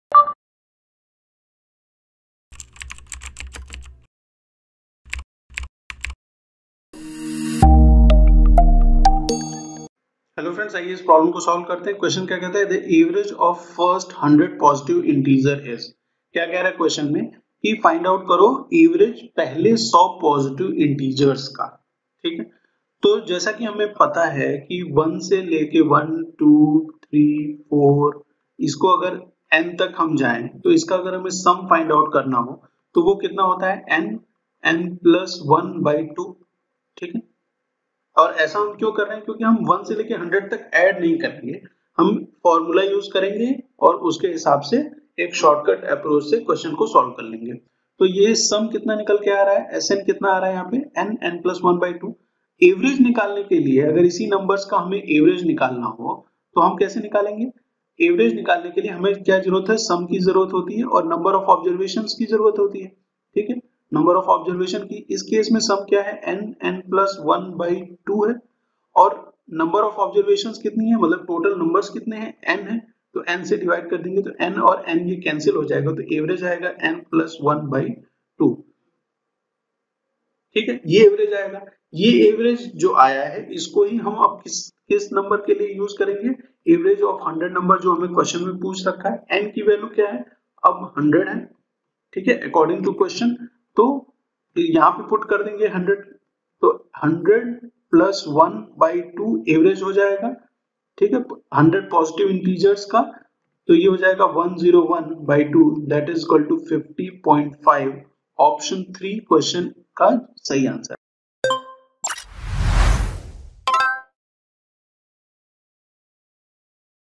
The average of the first 100 positive integers is (1) 100 (2) 51(3) 50.5 (4) 49.5 ?
The average of the first 100 positive integers is